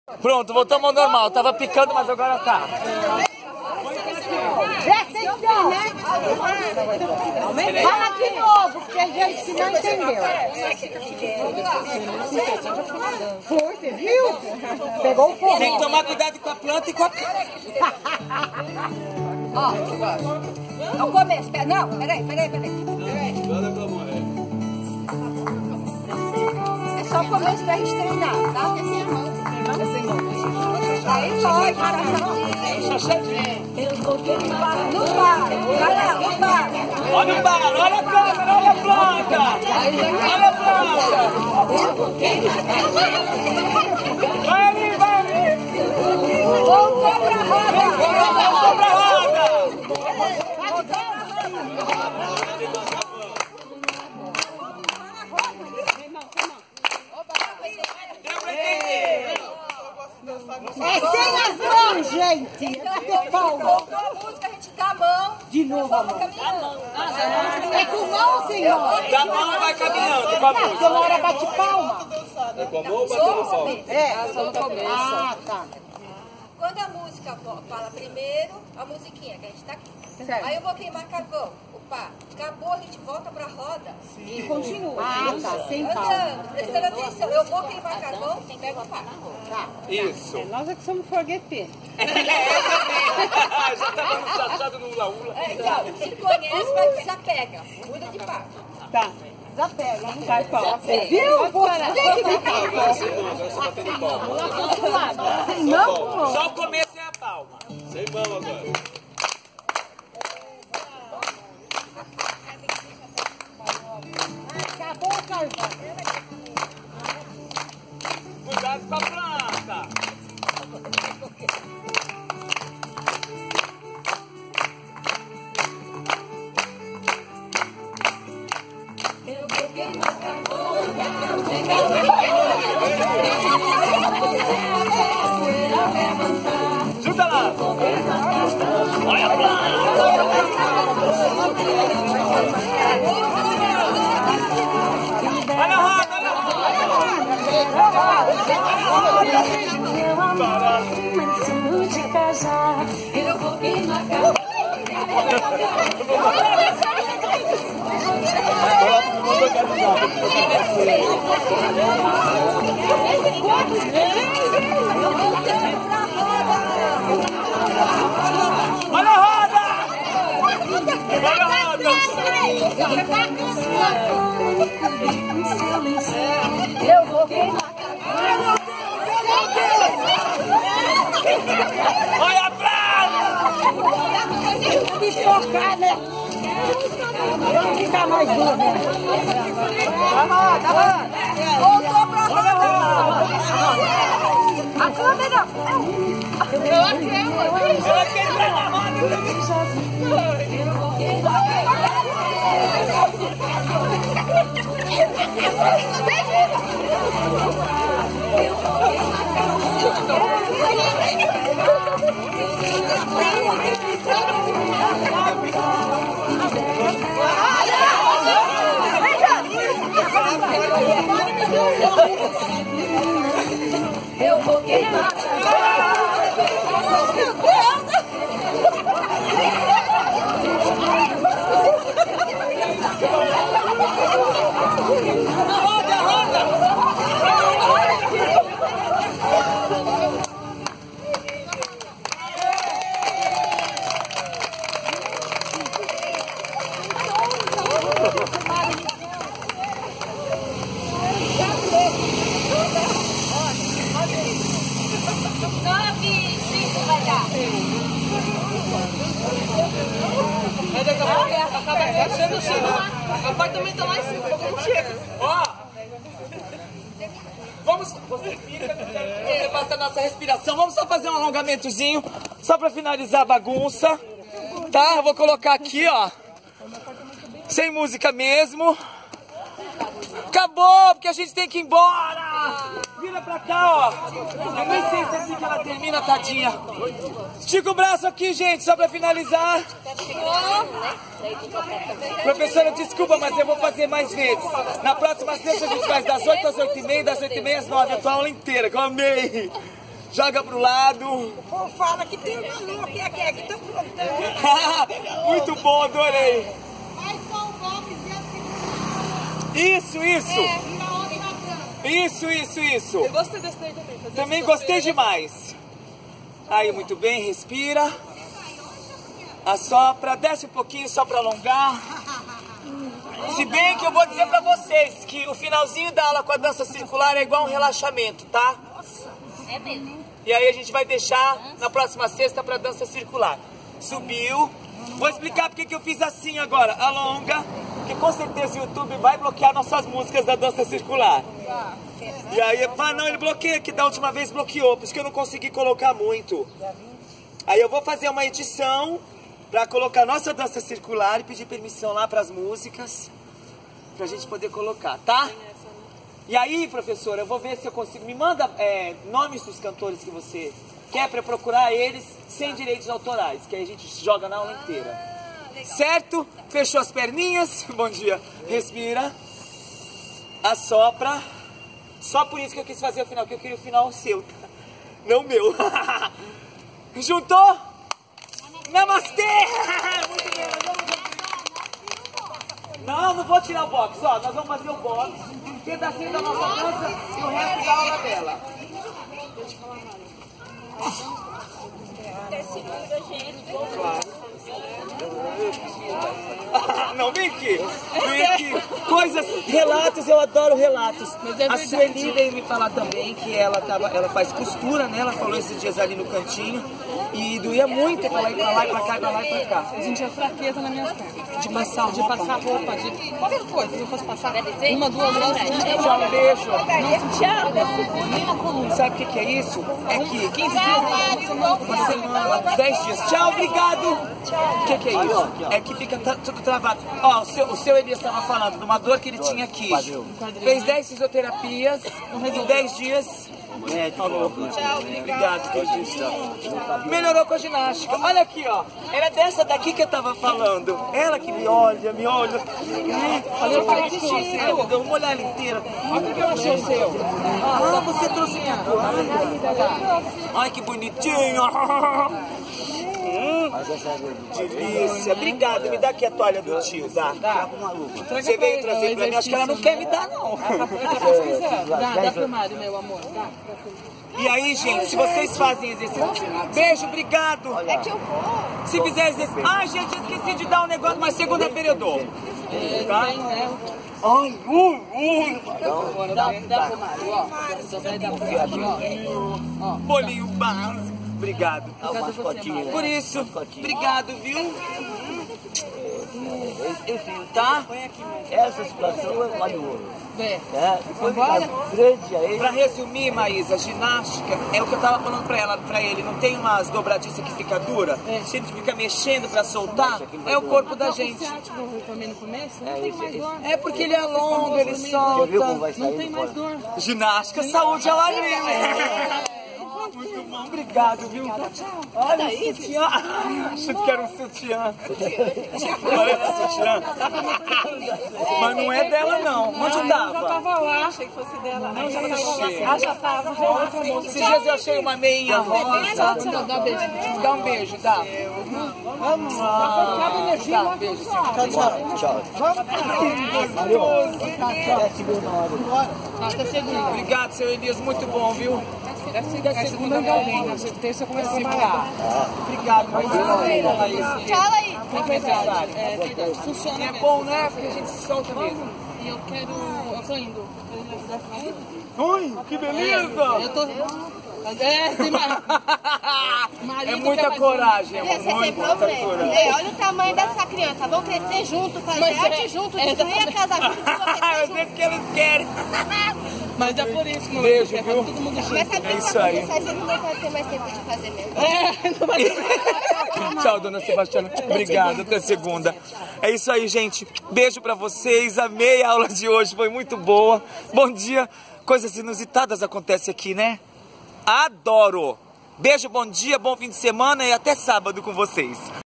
13/04/2018 - Dança circular hoje tambem teve...
Foi só um pedacinho para vcs, somente verem, e foi muito divertido venha vc tambem participar toda sexta... ameiiiii